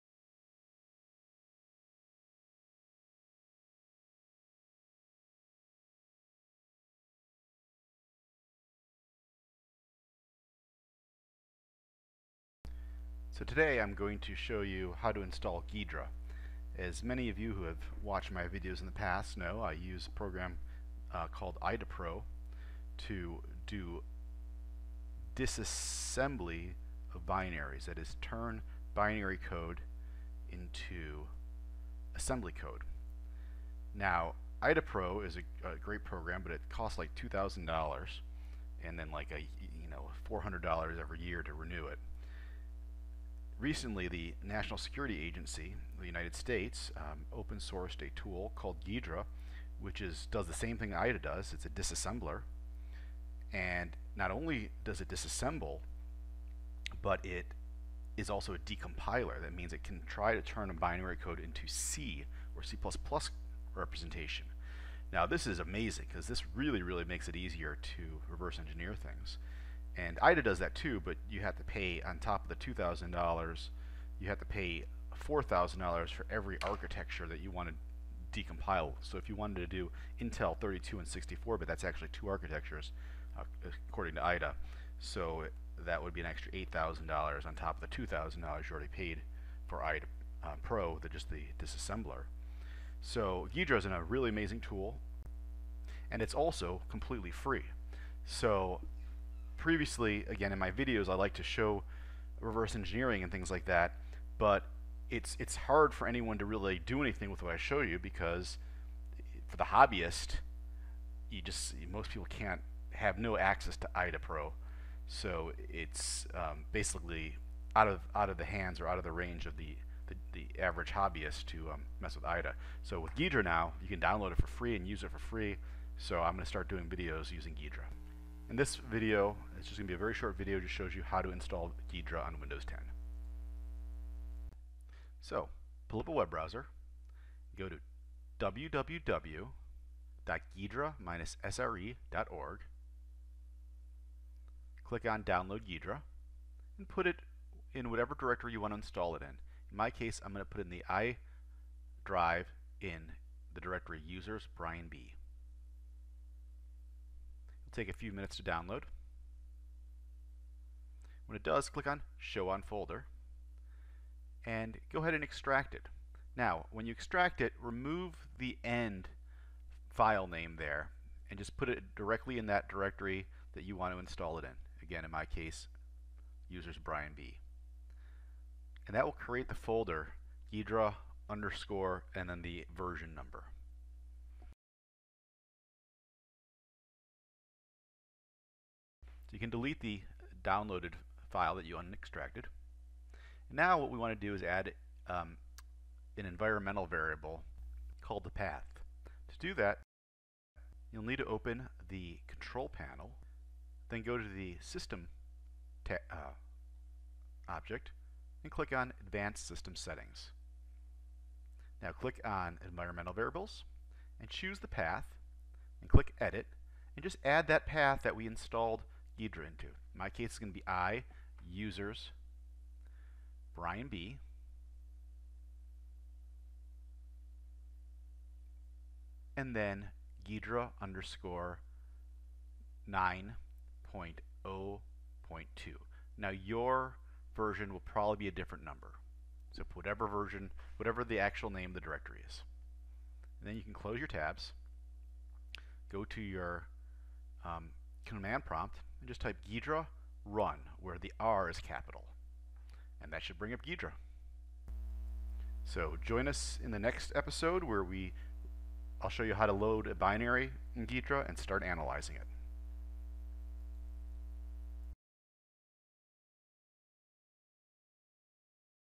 Installing Ghidra disassembler and decompiler
In this very short video I introduce you to a powerful new and FREE tool for reverse engineering. Ghidra which is a dissembler AND a decompiler. I must have for any reverse engineer, or arcade or console game hacker.